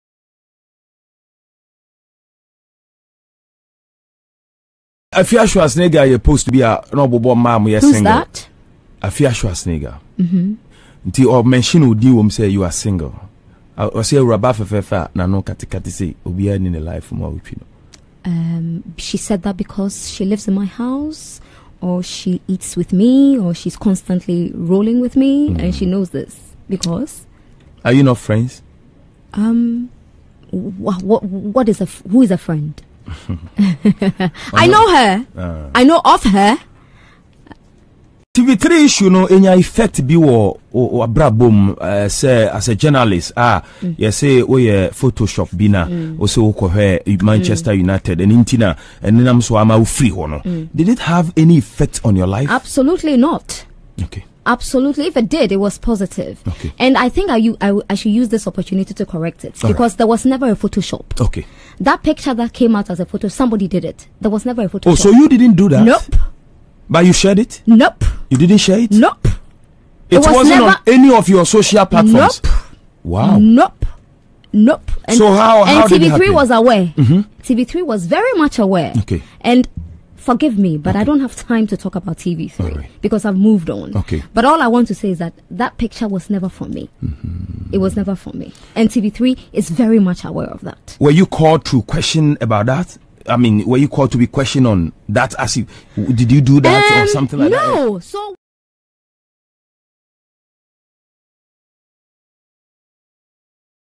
Nana Aba throws shades at Afia Schwarzenegger,says she doesn’t know who’s he is
Queen Elizabeth’s daughter @thenanaaba says she has heard of @queenafiaschwarzenegger but she’s not her friend . She asked what is Afia’s reason for saying she’s single ? Does she stay with me , Does she eat with me or is she even my friend ?